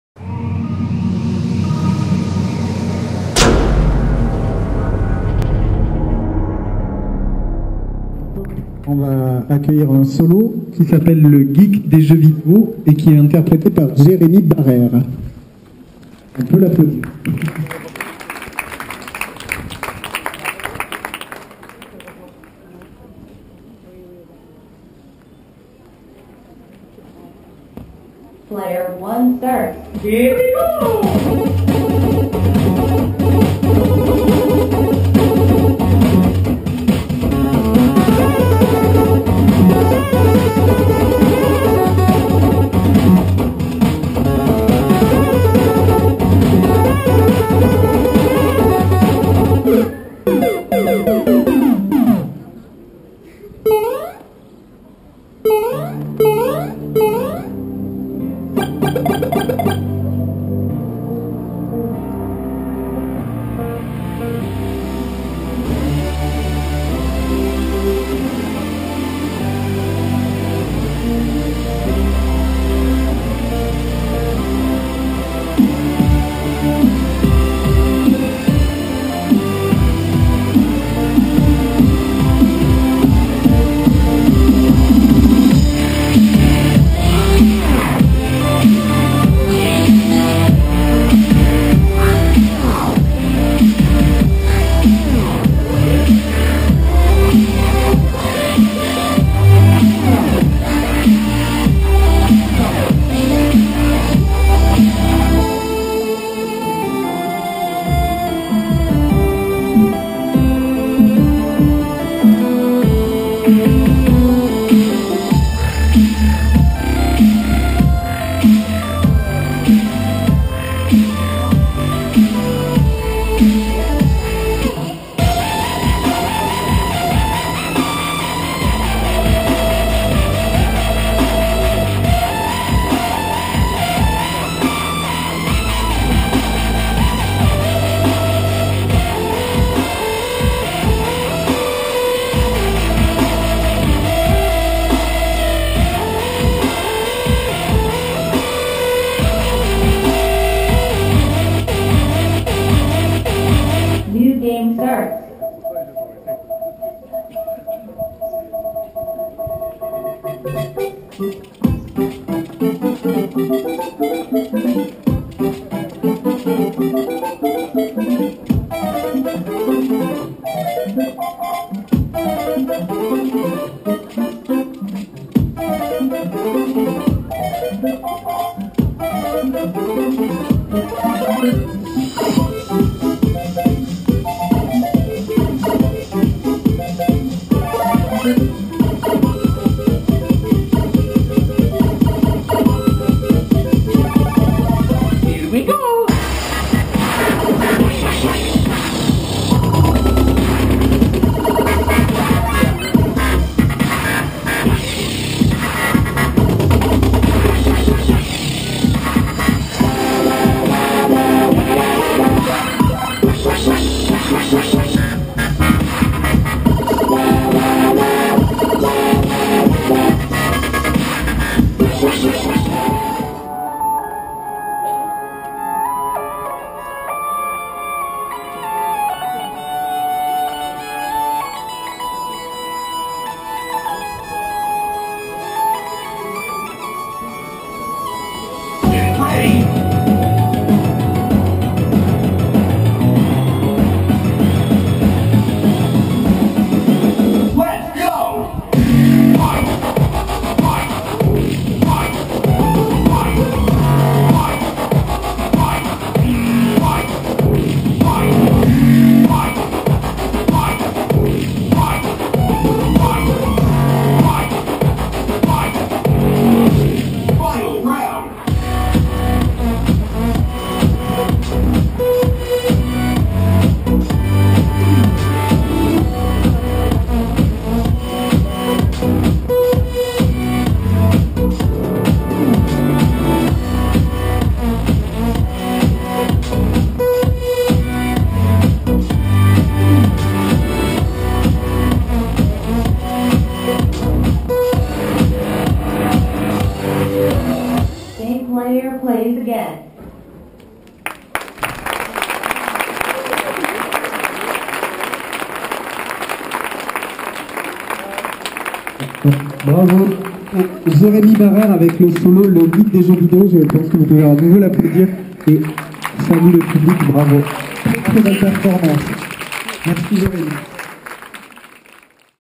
Modzii | Le geek des jeux vidéos
Hi everybody,

this is my first scene of the year 2014.

Dancing for Les estivales de la danse de Tarbes